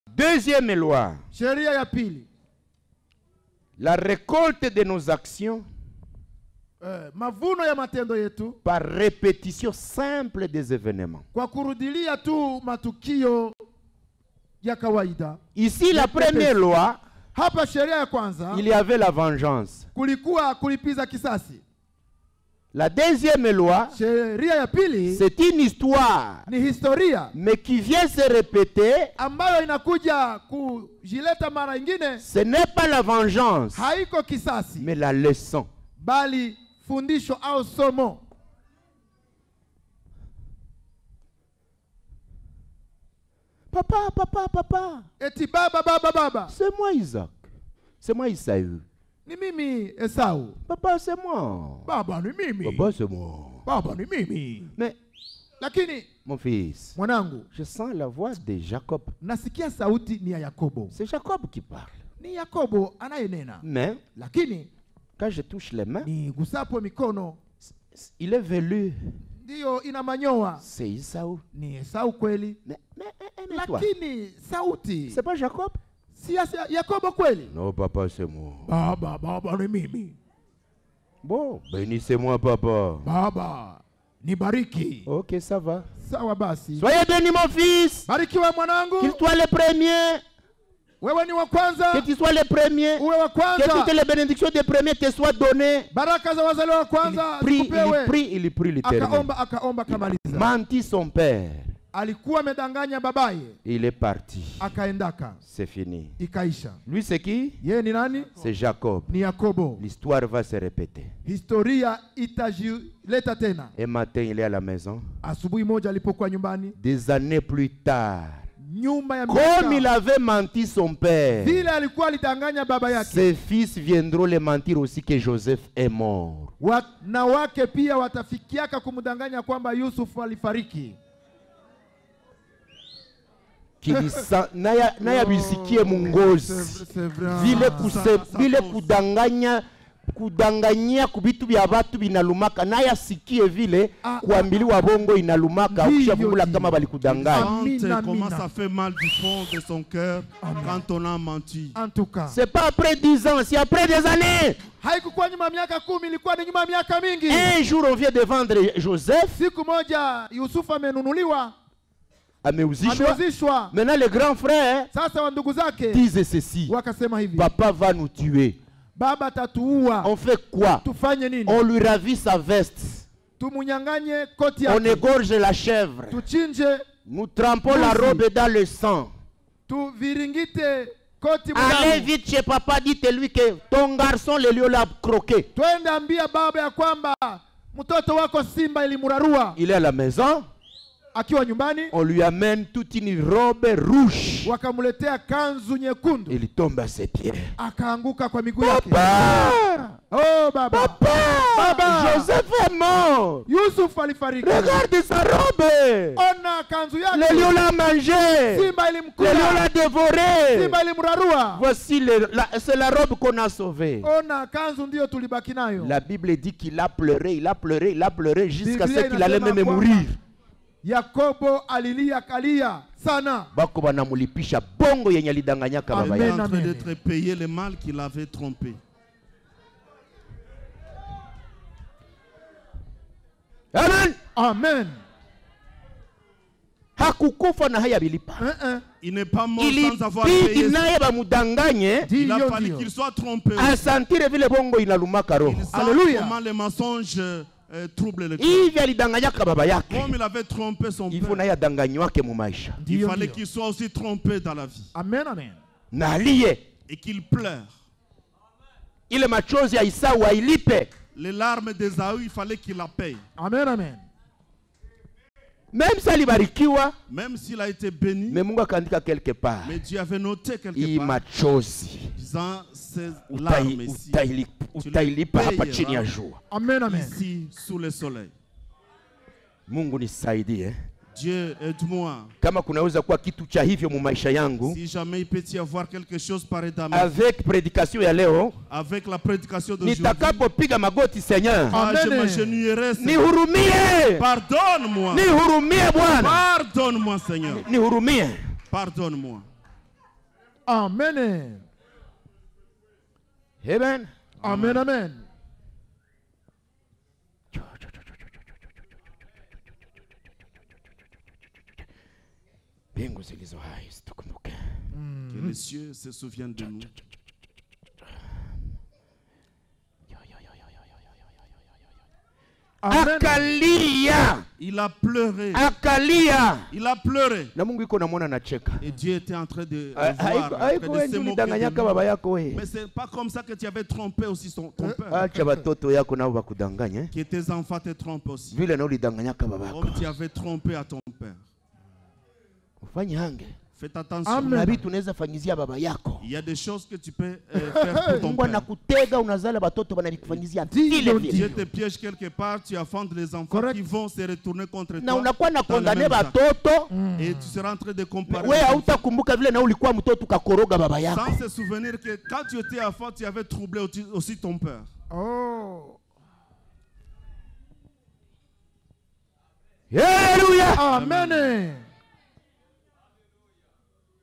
Loi de récolte 2: Répétition de l'histoire que tu fais vivre les autres.
La deuxième loi spirituelle traite de la manière dont Dieu accomplit Sa vengeance à travers la répétition des actions humaines. Cette loi illustre comment les actions de trahison, de mensonge ou d'injustice finissent par se retourner contre ceux qui les commettent. Le récit biblique de Jacob, qui a trompé son père Isaac pour obtenir la bénédiction destinée à Ésaü, et de ses fils qui ont trompé Jacob au sujet de la disparition de Joseph, en est un exemple. Dieu permet que les actions malveillantes se reproduisent dans l'histoire, assurant ainsi que la justice divine soit rendue.

Références Bibliques:

Genèse 27:21-24 : Jacob trompe Isaac pour obtenir la bénédiction d'Ésaü.
Genèse 37:27-36 : Les fils de Jacob le trompent en simulant la mort de Joseph.